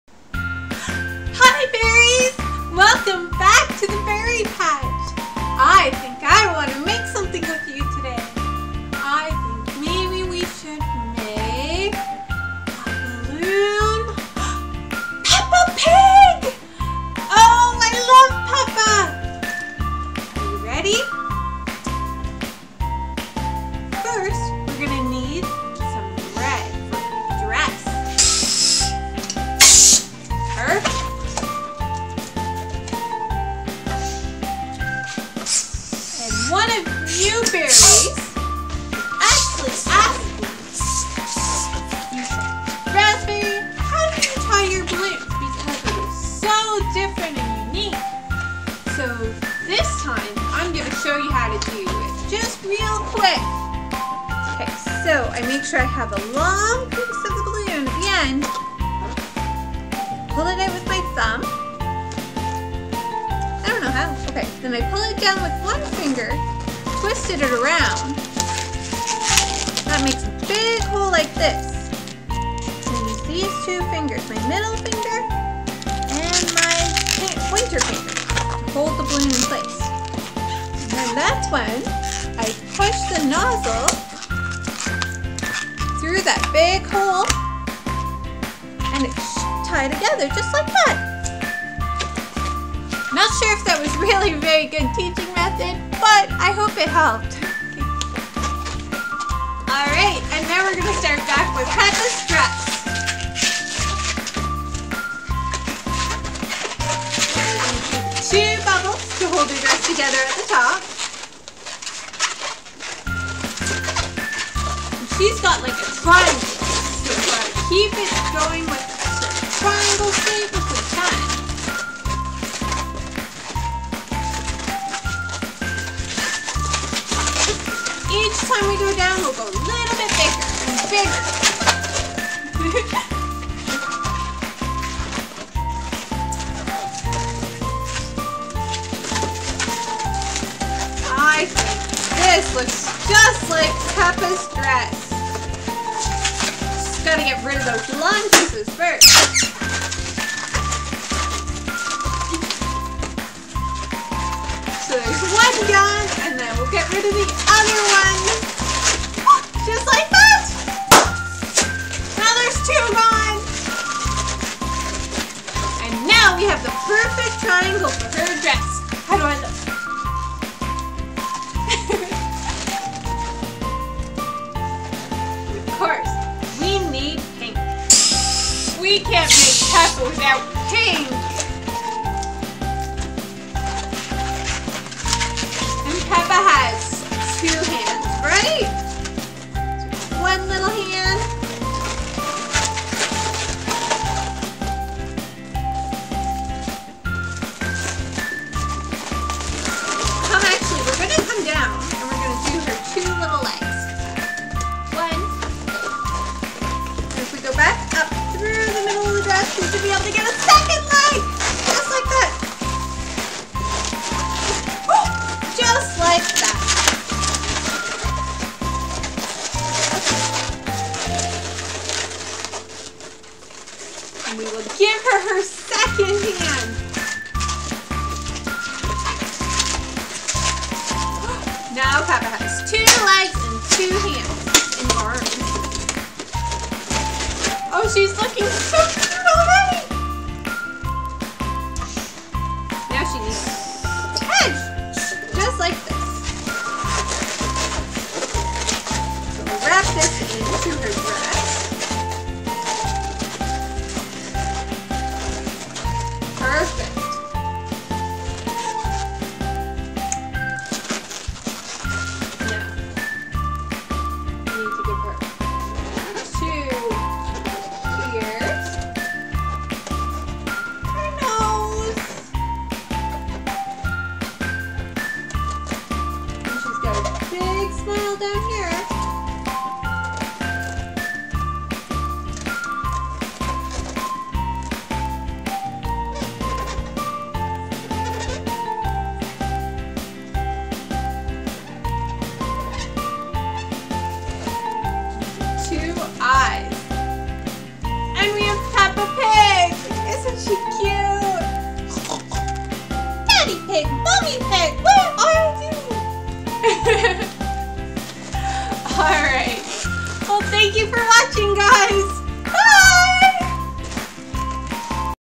Making a Peppa Pig balloon with Razberry
Lets me a Peppa Pig balloon together.
in this video i used
2 red 260's
1 pink 260
and 1 pink heart

let me know if you think this looks like Peppa Pig